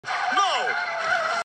Am I Ever Going Back To Liking MrAcrizzy?
NO!

Because Of What He Did Back in 2020